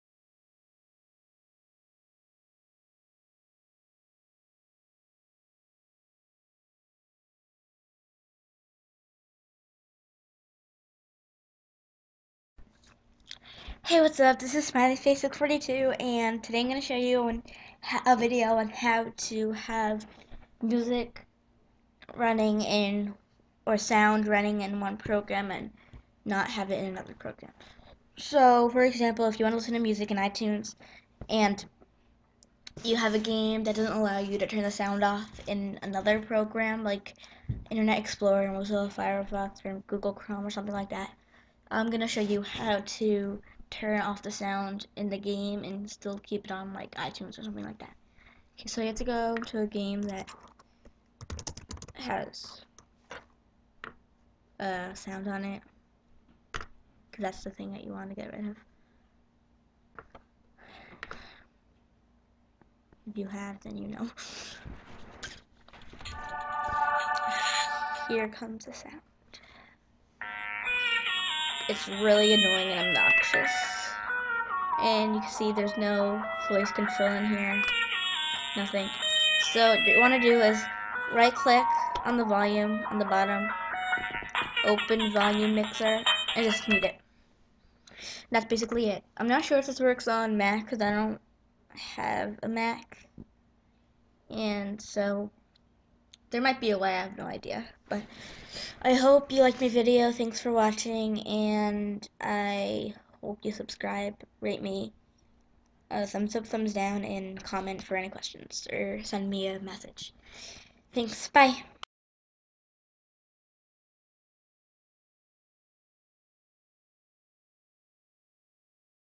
how to listen to sound in one program and mute it in another
This is how to mute one program but keep another one  running.